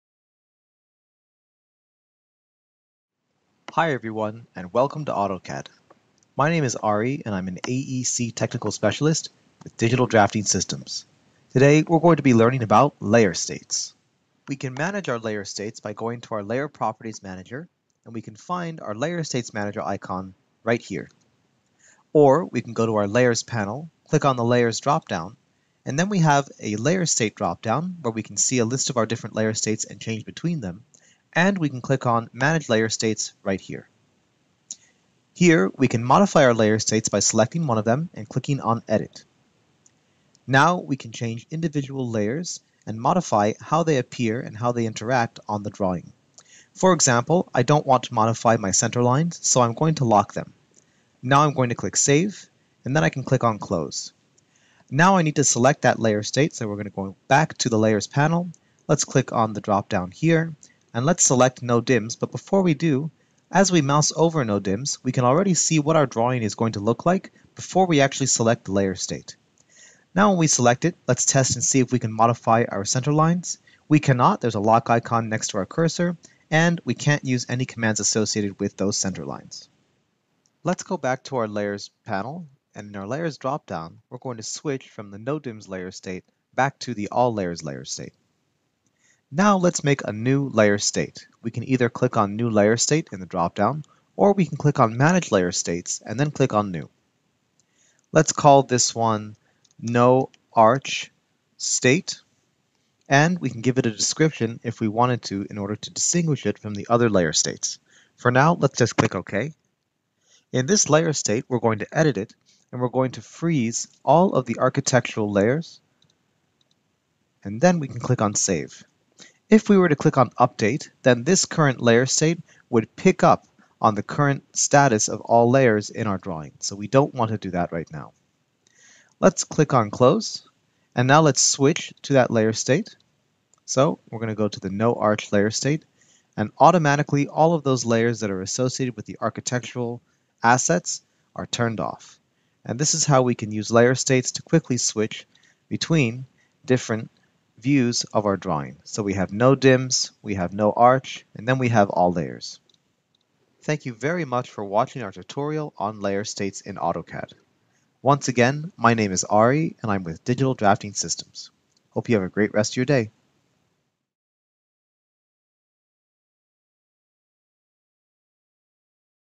Layer States with AutoCAD
Join our AEC Technical Specialist, Ari Rejtman, as he talks about layer states in AutoCAD.